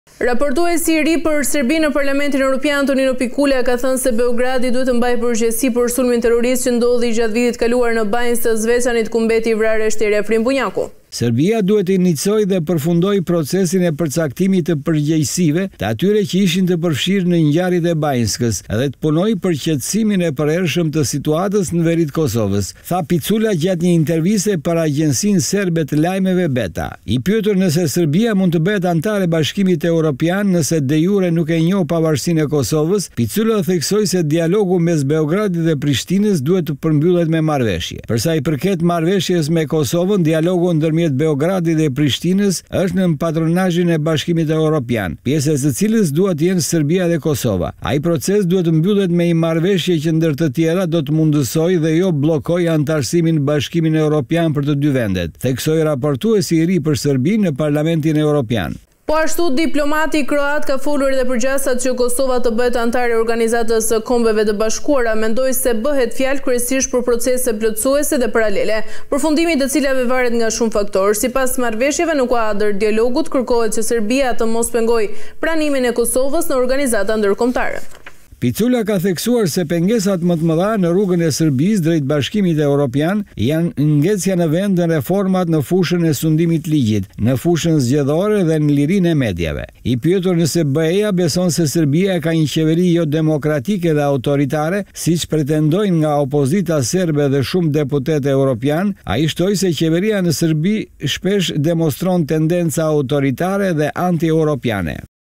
SERBIA NË GJYKIM HISTORIK/ Picula shpërthen: Krimet e Banjskës janë plagë që s’mbyllen kurrë!
PASOJAT TRAGJIKE PER KOSOVEN
ÇFARE PO PARALAJMERON TRAJKOVIÇ?
VJEN “GODITJA” NGA BISERKO
SERBIA PAGUAN “KRIMET” QE KA BERE
“URDHERI” I MADH NGA TRUMP

𝐀𝐮𝐭𝐨𝐫 : NTV